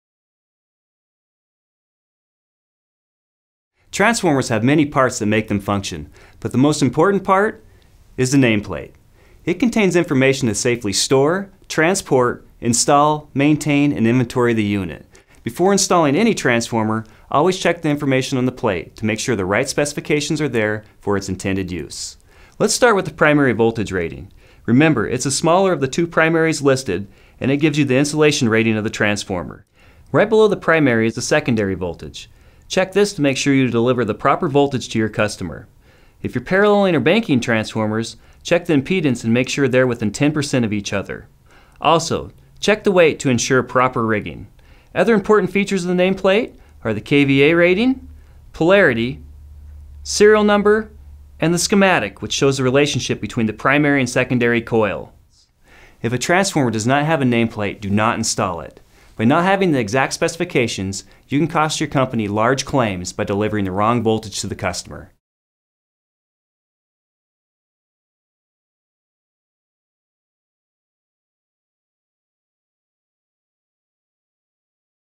Transformer Nameplates
Before installing a transformer, a nameplate must be examined to make sure it is correct for the intended use. Learn the components of and how to properly read a transformer nameplate in this video.